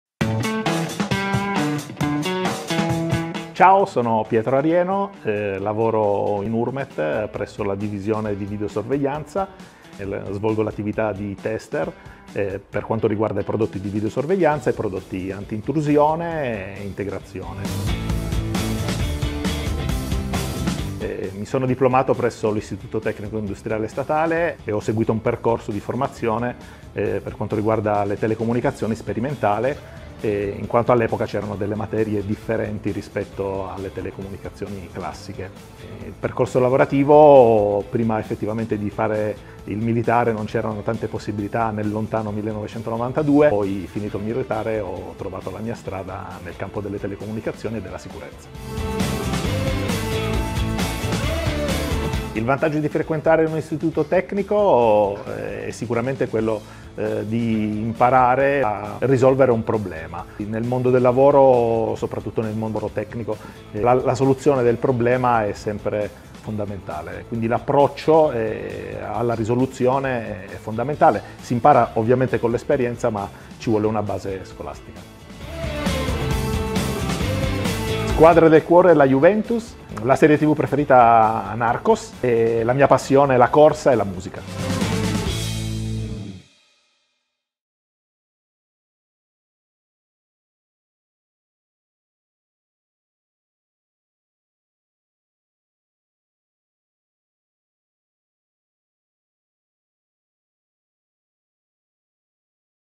Urmet Class - episodio 1. Pietro Arieno
Dai banchi di scuola fino al mondo del lavoro: Urmet Class racconta l’iter scolastico di alcuni dei professionisti dell'azienda. Nel primo episodio vi presentiamo Pietro Arieno, che oggi è specializzato in test e valutazione dei sistemi di videosorveglianza e antintrusione, punto di riferimento nel settore sicurezza in Urmet, formatosi presso l'Istituto Tecnico Industriale Bodoni di Torino.  Guarda la sua storia!